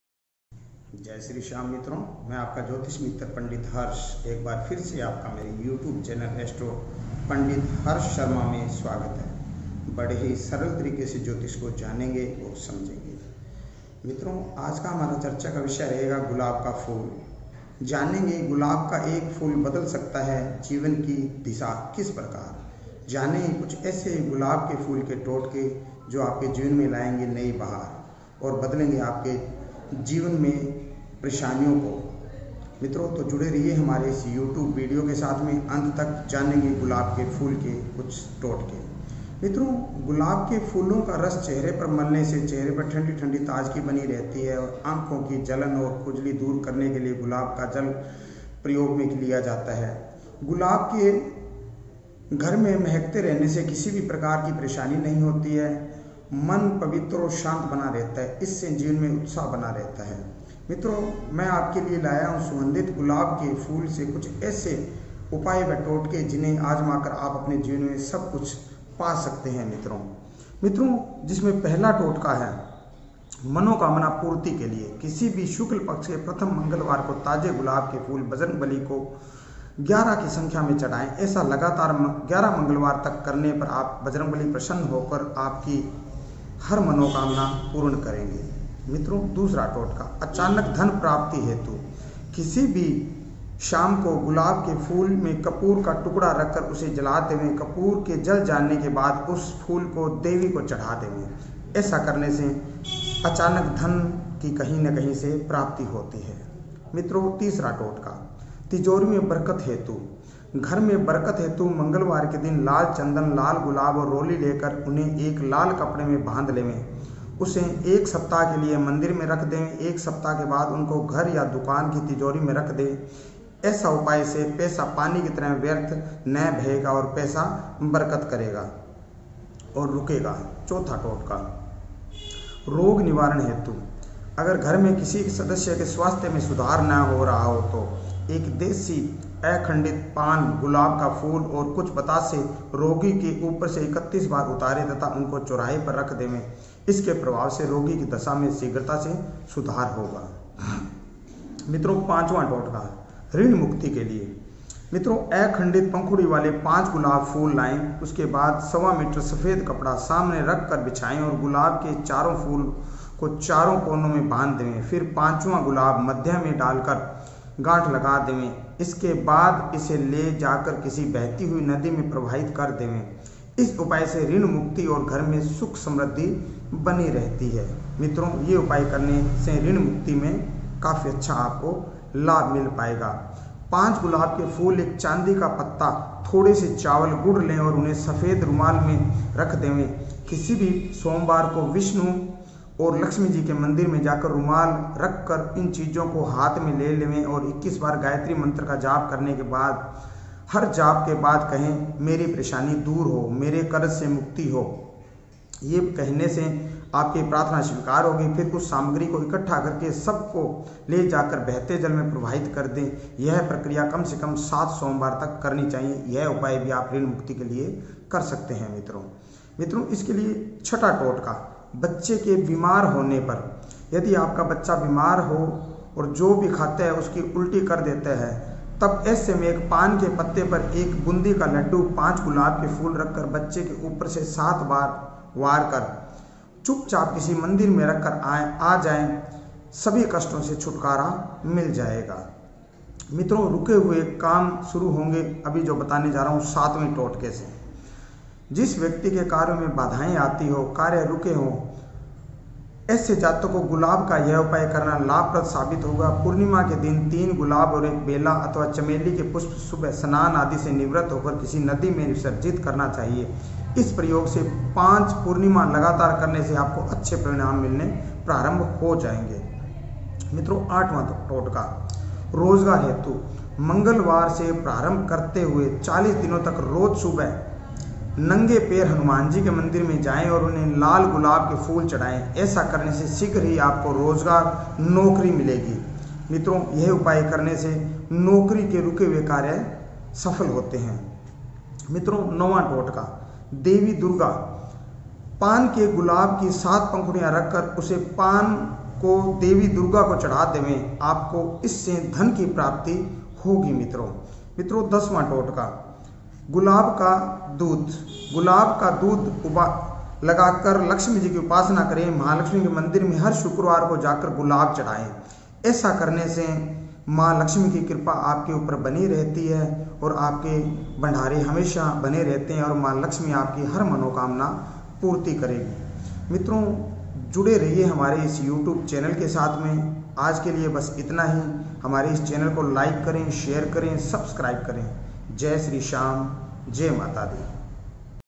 गुलाब के फूल के 10 अचूक उपाय आपके खोलेंगे धन प्राप्ति के मार्ग आइए जानते हैं !
आपका ज्योतिष मित्र